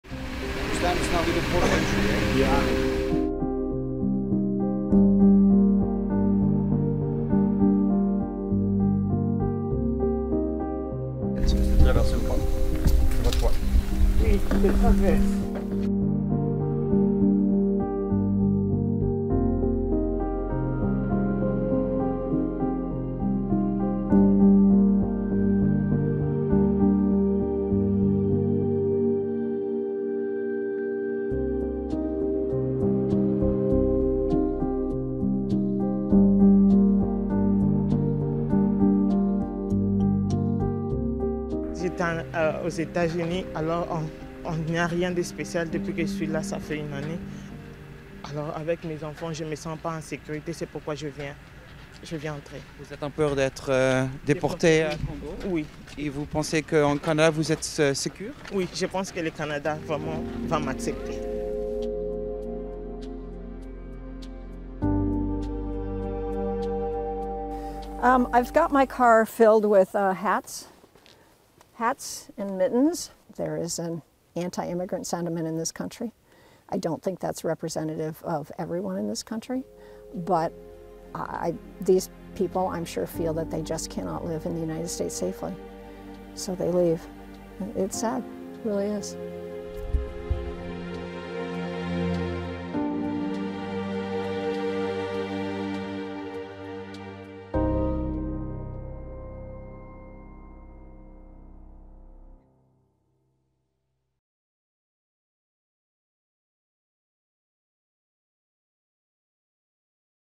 Asielzoekers in VS naar Canada uit angst voor Trumps immigrateibeleid - RTL NIEUWS
Bij een onofficiële grensovergang van de Verenigde Staten proberen vluchtelingen Canada te bereiken. Een Congolese vrouw met haar kinderen hoopt daar meer geluk te hebben: ‘Ik ben al een jaar in de VS en er gebeurt niets.’